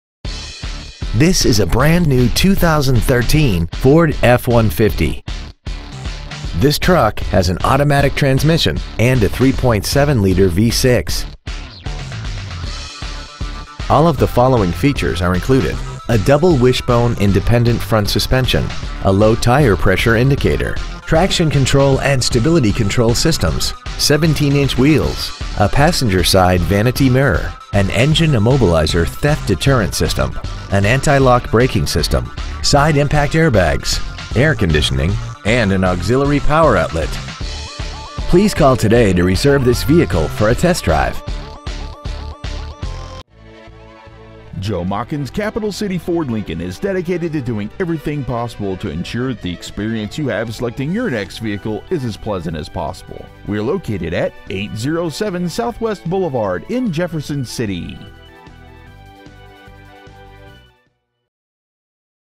2013 Ford F-150 Jefferson City MO
Year : 2013
 Make : Ford
 Model : F-150
 Engine : 3.7L V6 24V MPFI DOHC FLEXIBLE FUEL
 Trans . : 6-SPEED AUTOMATIC
 Exterior : OXFORD WHITE

 Interior : GRAY
 Stock : 31752

 807 Southwest Blvd.